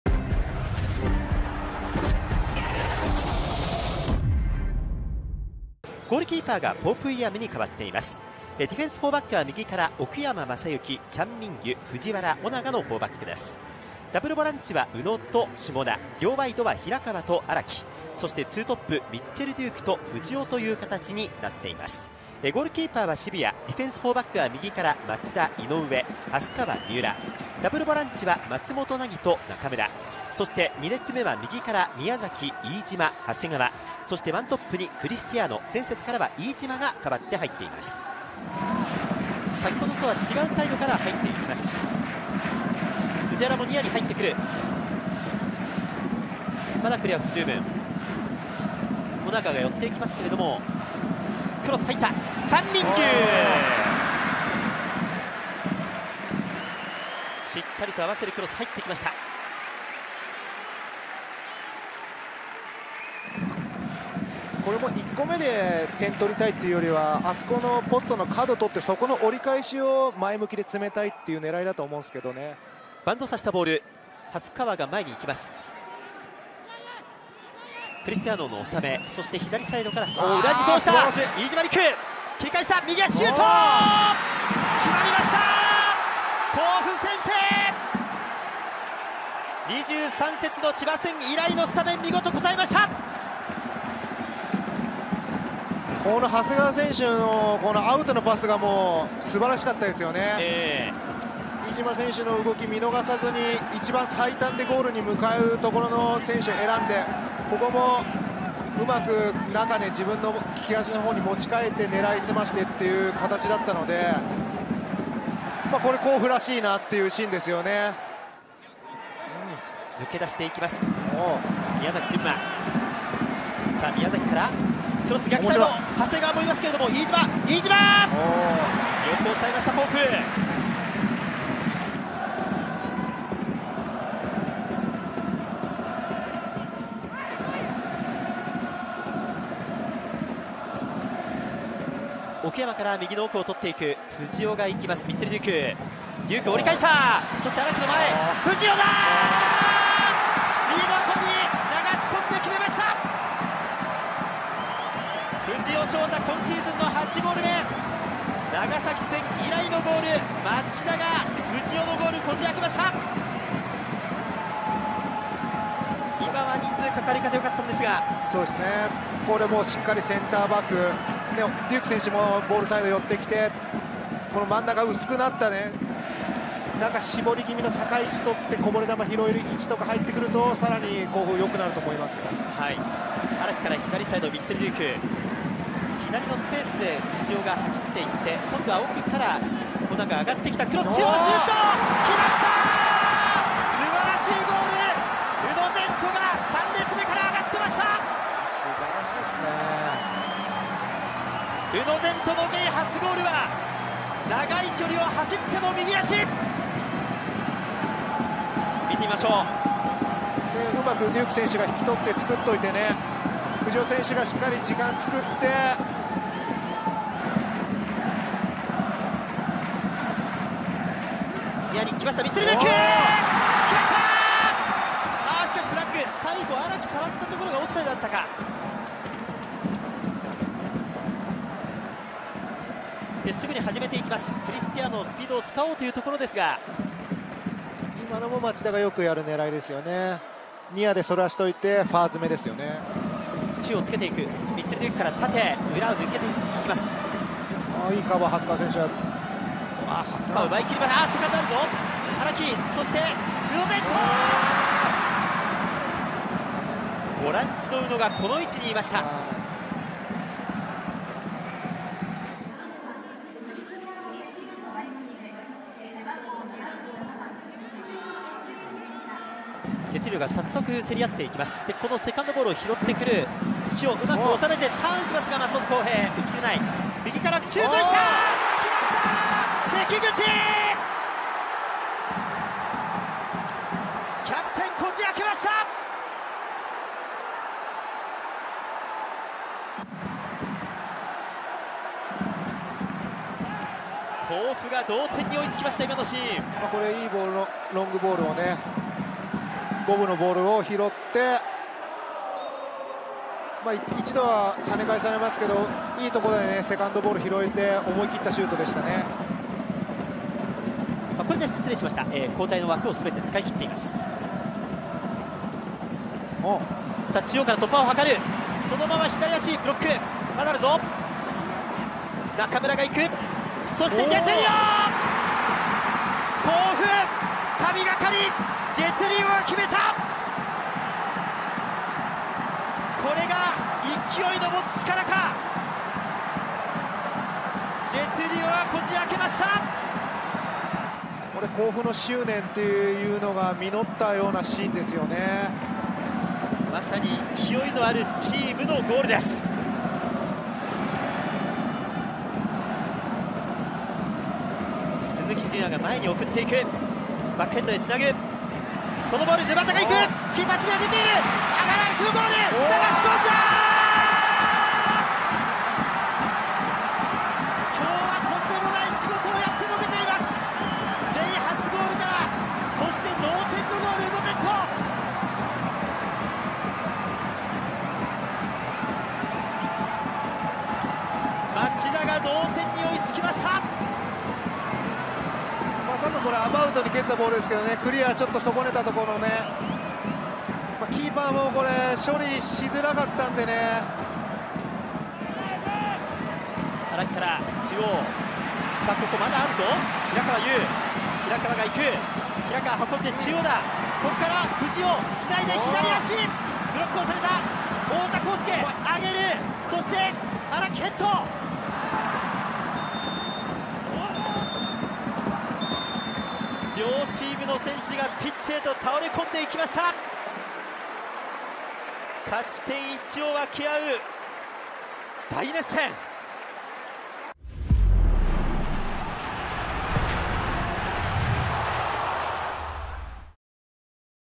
【ハイライト｜3-3 FC町田ゼルビア vsヴァンフォーレ甲府｜Jリーグ】
【2023明治安田生命J2リーグ第38節 ハイライト動画】

0:06 両チームスタメン紹介
0:39 0-0 試合開始
1:24 0‐1 分 飯島陸(甲府)
2:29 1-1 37分 藤尾翔太(町田)
3:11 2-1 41分 宇野禅斗(町田)
4:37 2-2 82分 関口正大(甲府)
5:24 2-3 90分₊2分 ジェトゥリオ(甲府)
6:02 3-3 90分₊7分 宇野禅斗(町田)
6:53 3-3 試合終了

ポープ　ウィリアム
奥山　政幸
チャン　ミンギュ
藤原　優大
翁長　聖
平河　悠
宇野　禅斗
下田　北斗
荒木　駿太
藤尾　翔太
ミッチェル　デューク

▼サブ
福井　光輝
太田　宏介
鈴木　準弥
稲葉　修土
安井　拓也
バスケス　バイロン
沼田　駿也

▼監督
黒田　剛